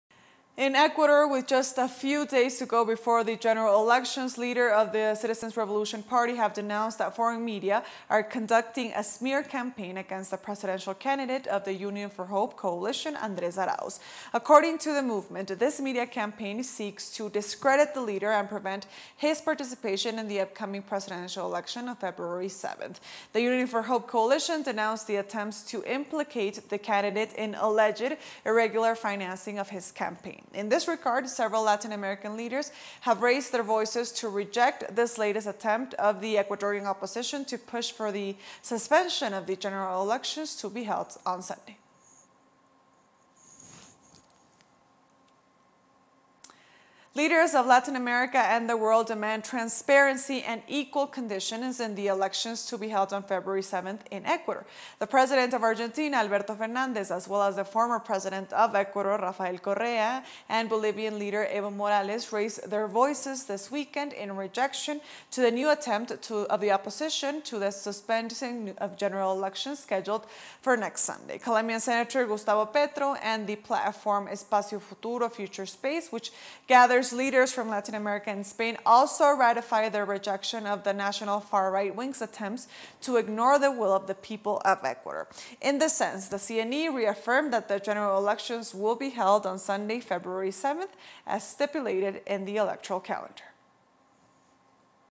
Opposition seeks the suspension of the presidential elections in Ecuador
While a media campaign targets Ecuadorian Union for Hope candidate, the movement denounced the attempts to implicate Andres Arauz in irregular financing of his campaign, just a few days before the elections scheduled next February 7th. teleSUR
This and many other stories now!